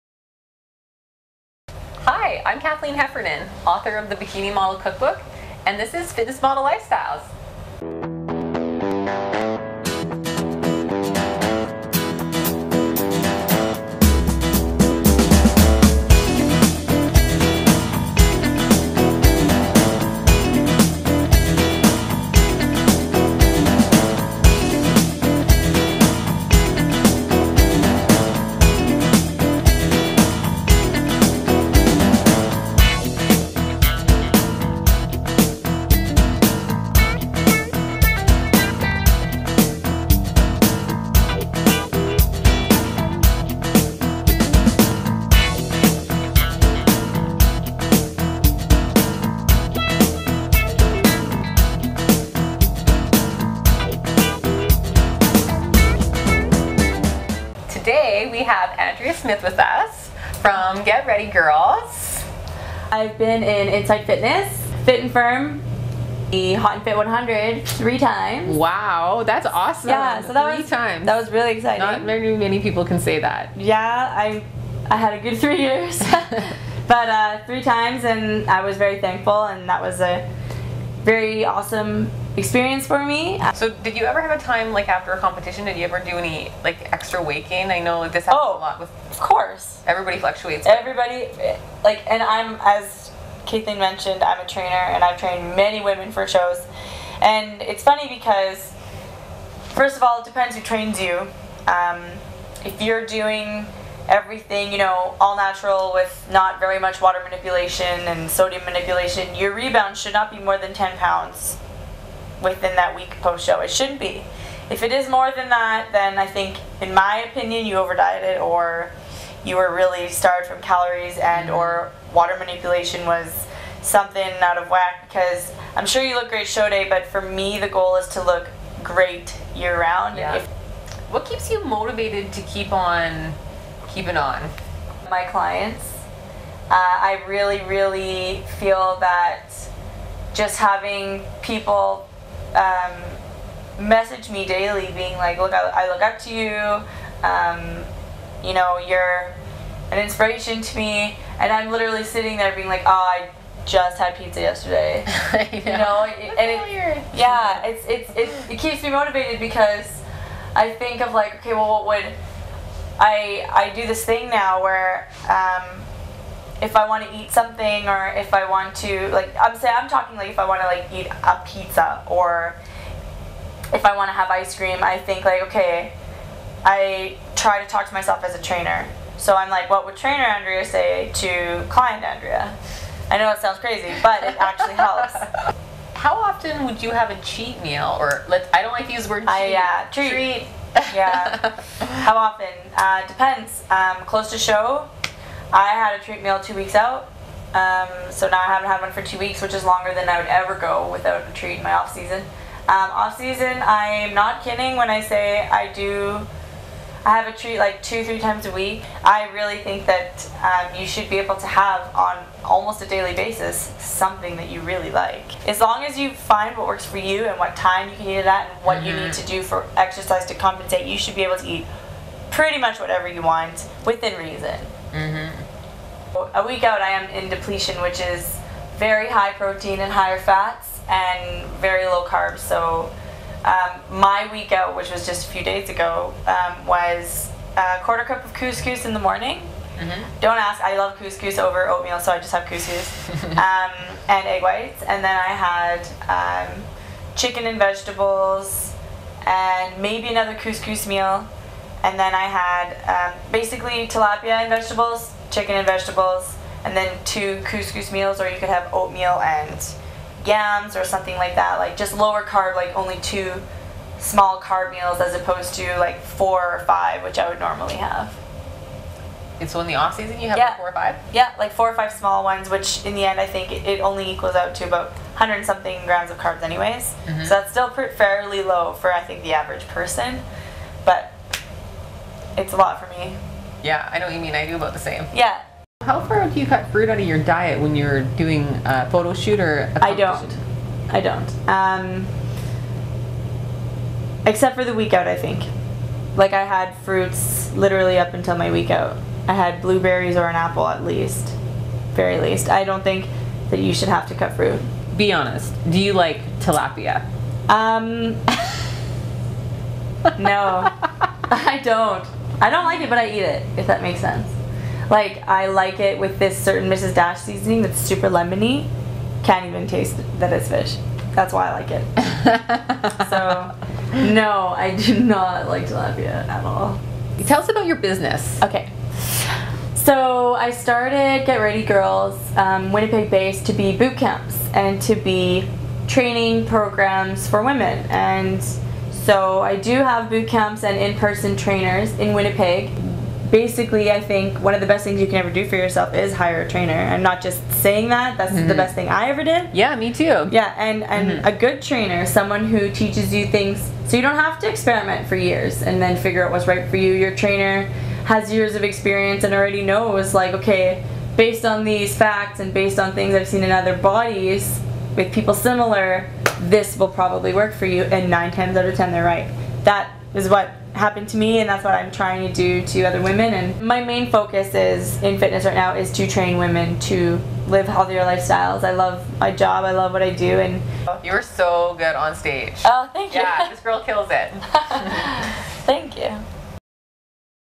Fitness Model Lifestyle Interview with Bikini Model Andrea Smith
I had a great visit with Andrea Smith. This girl competes at the PRO level and is a Cover Model of Shape Magazine. For the cookbook and more on Andrea visit this
or checkout for the full interview with details on how Andrea eats clean and trains for best results.